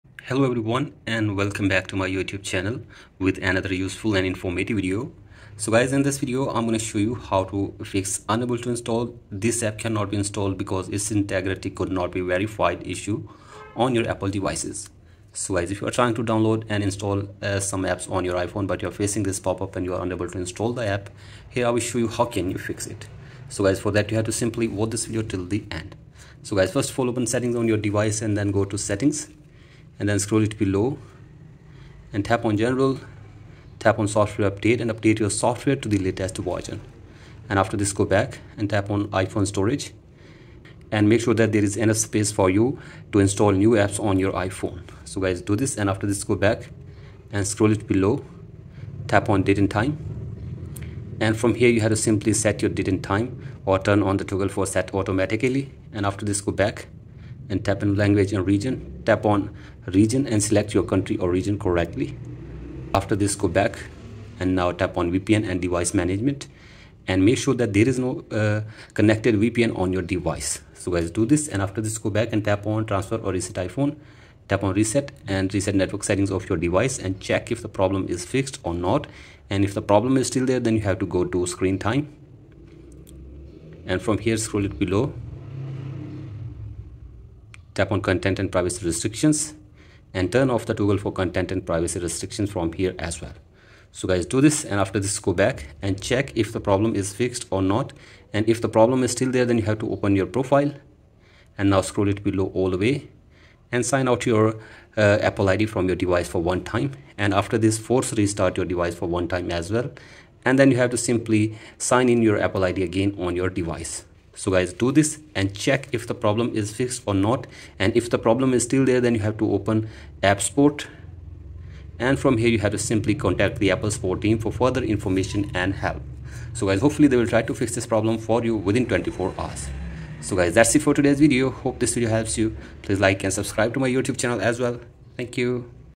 How To Fix This App Cannot be Installed because its integrity could not be Verified / 2025
Are you getting the error: "This App Cannot Be Installed Because Its Integrity Could Not Be Verified" on your iPhone or iPad in 2025? 😤 Don’t worry — this step-by-step guide will help you fix it in minutes!

In this video, I’ll walk you through all the possible solutions to fix the integrity verification error on iOS 17 and iOS 18. Whether you're using AltStore, TestFlight, or sideloading apps with third-party tools, these fixes work!